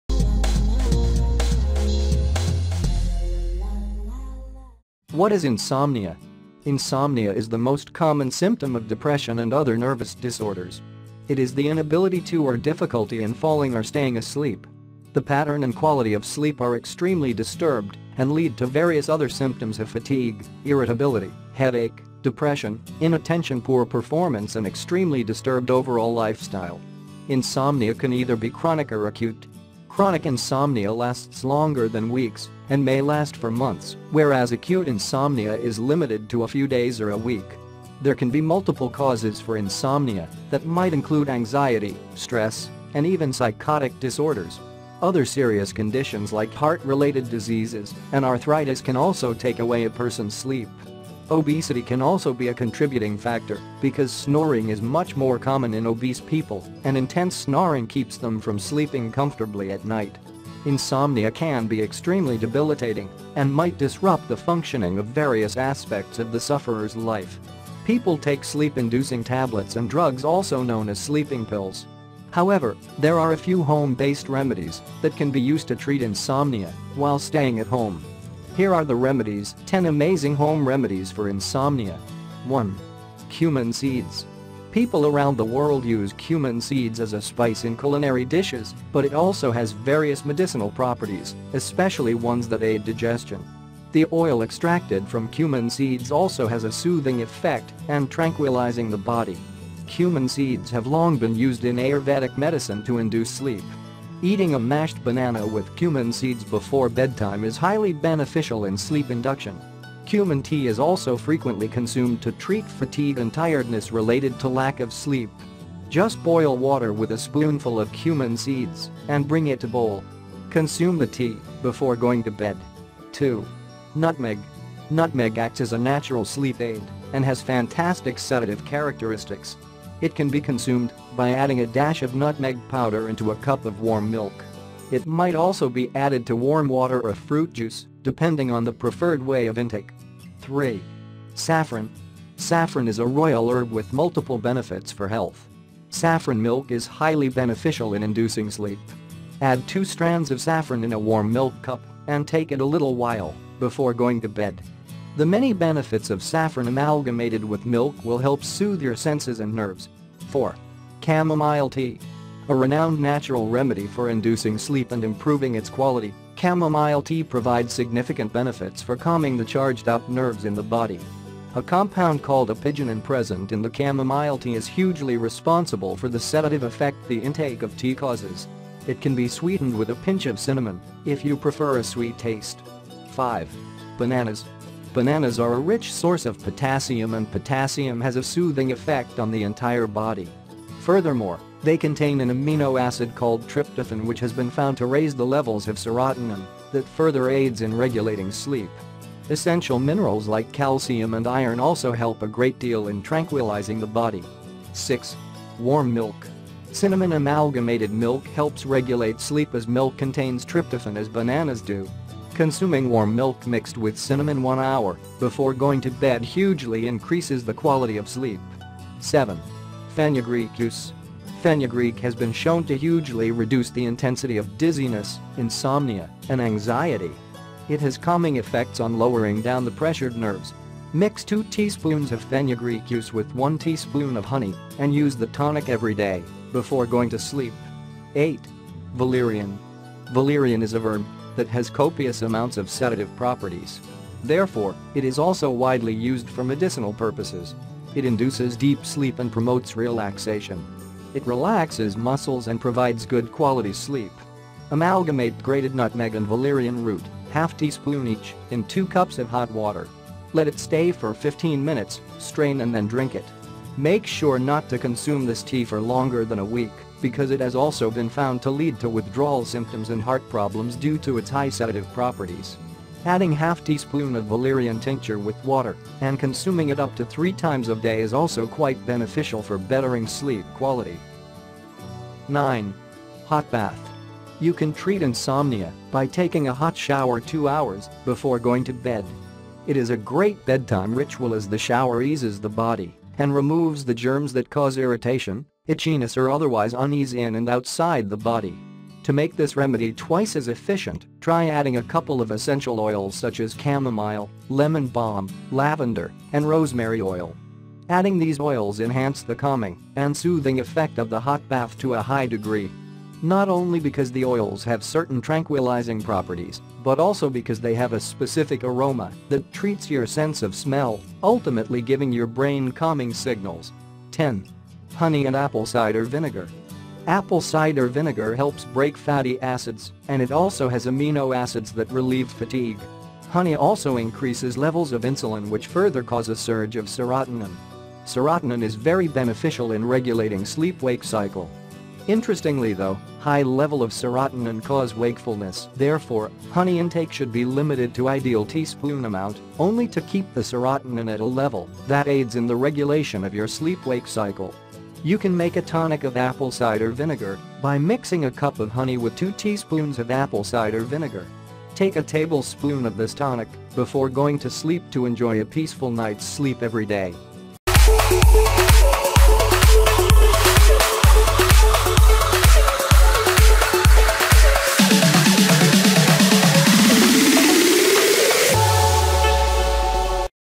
10 Amazing Home Remedies For Insomnia
10 Amazing Home Remedies For Insomnia

What is Insomnia?
Insomnia is the most common symptom of depression and other nervous disorders. It is the inability to or difficulty in falling or staying asleep. The pattern and quality of sleep are extremely disturbed and lead to various other symptoms of fatigue, irritability, headache, depression, inattention poor performance and extremely disturbed overall lifestyle.

Insomnia can either be chronic or acute. Chronic insomnia lasts longer than weeks and may last for months whereas acute insomnia is limited to a few days or a week. There can be multiple causes for insomnia that might include anxiety, stress, and even psychotic disorders. Other serious conditions like heart related diseases and arthritis can also take away a person’s sleep. Obesity can also be a contributing factor because snoring is much more common in obese people and intense snoring keeps them from sleeping comfortably at night.

Insomnia can be extremely debilitating and might disrupt the functioning of various aspects of the sufferer’s life. People take sleep-inducing tablets and drugs also known as sleeping pills. However, there are a few home based remedies that can be used to treat insomnia while staying at home. Here are the remedies: